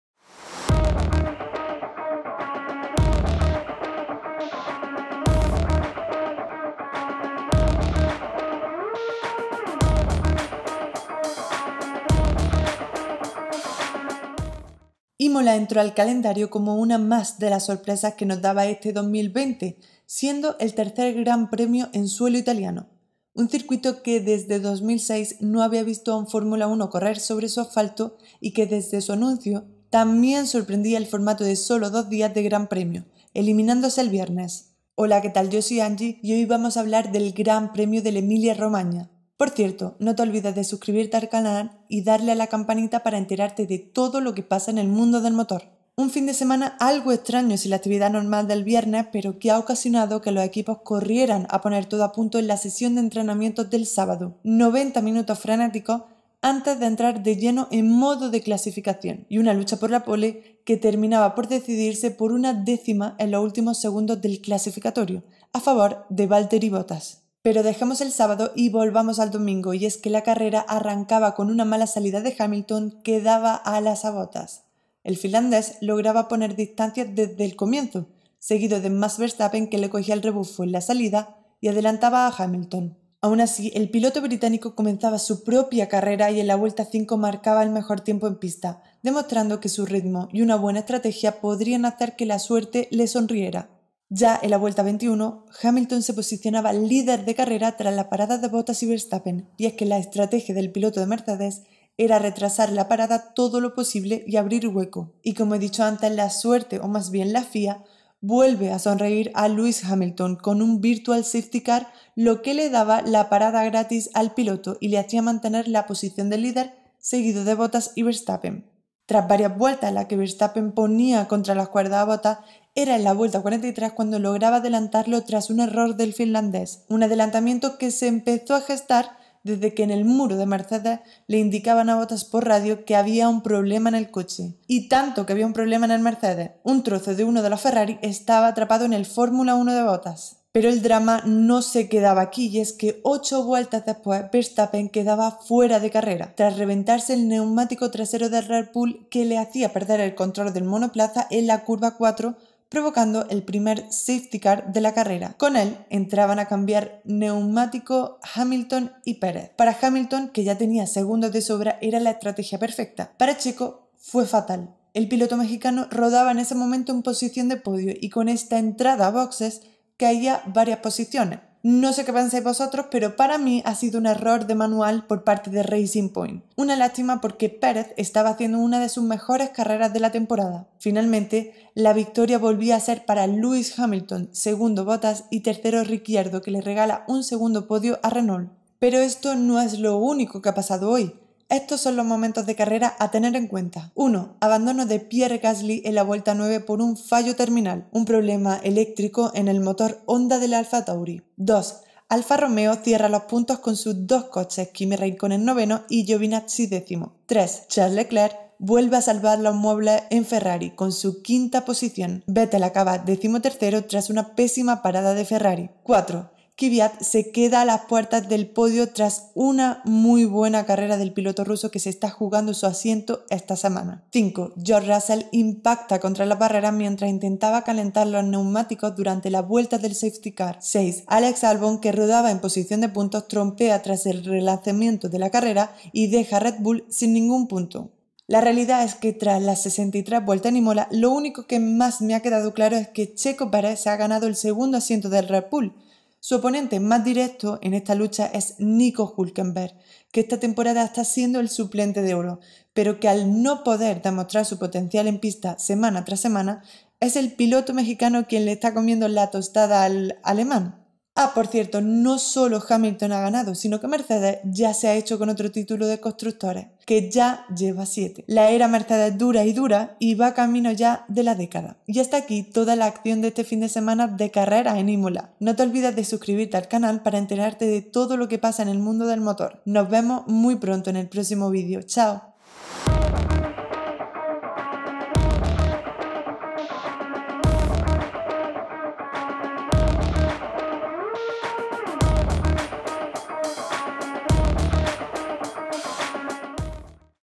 Hamilton gana en Imola y Mercedes se hace con el Título de Constructores | F1 2020
Que levante la mano quien crea que Checo Perez se ha ganado el puesto en Red Bull 🙋🙋🙋

El Gran Premio de'll Emilia Romagna de Formula de este domingo corona a Mercedes en el campeonato de constructores:
* Hamilton vuelve a ganar
* Verstappen queda fuera de carrera
* Ricciardo y Renault vuelven al podio
* ¿Racing Point le ha robado el podio a Checo Perez?

Este video está editado y grabado por mi. GRACIAAAAS